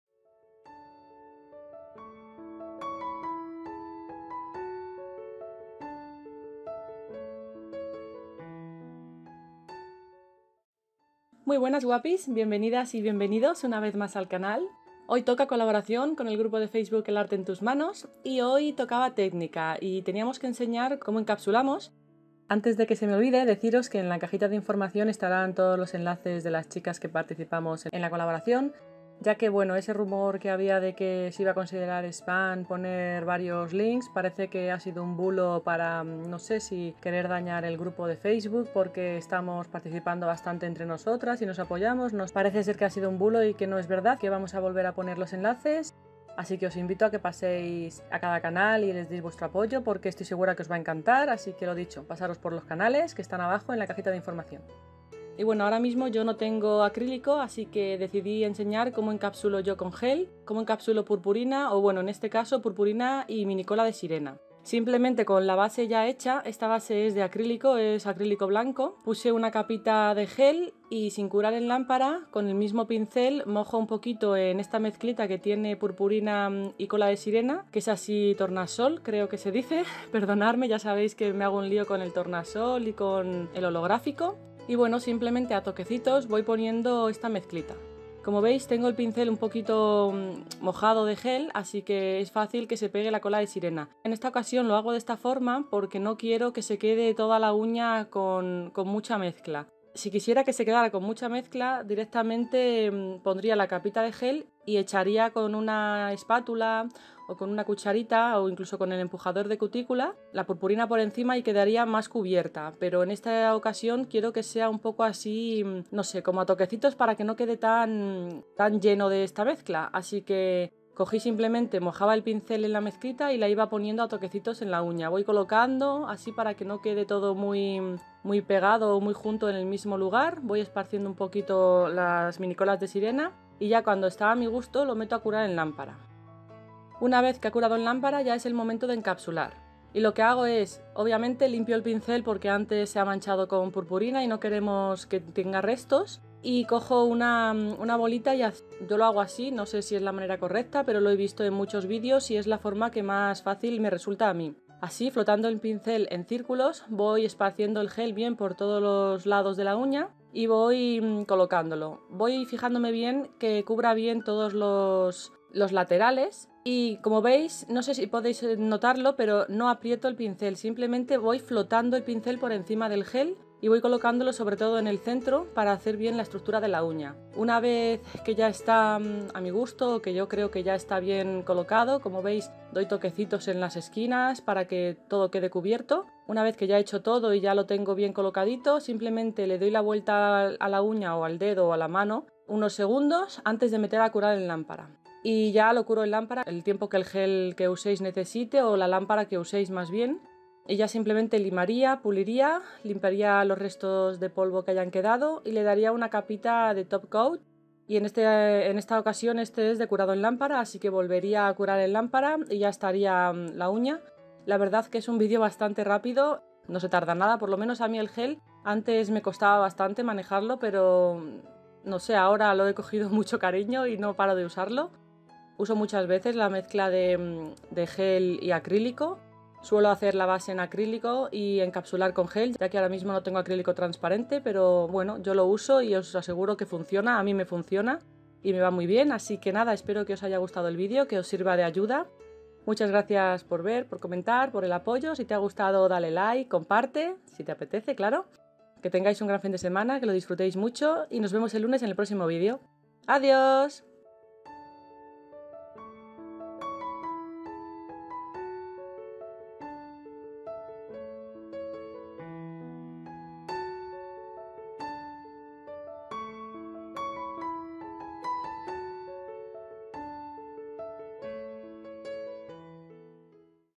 Como Encapsular Purpurina Con Gel / How to Encapsulate Glitter with Gel - Colaboración | mmariaa MK
Espero que os guste ^_^ Muakk 😘😘

Soy la creadora de todo el contenido de mis videos,
por lo que me reservo todos los derechos.